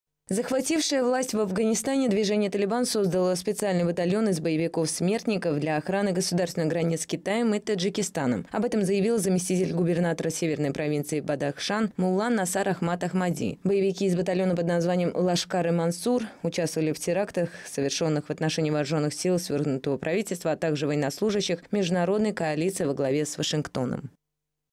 Охрана границ на севере Афганистана
Захватившее власть в Афганистане движение «Талибан» создало специальный батальон из боевиков-смертников для охраны государственных границ с Китаем и Таджикистаном. Об этом заявил заместитель губернатора северной провинции Бадахшан мулла Нисар Ахмад Ахмади. Боевики из батальона под названием «Лашкар-е Мансур» участвовали в терактах, совершенных в отношении вооруженных сил свергнутого правительства, а также военнослужащих международной коалиции во главе с Вашингтоном.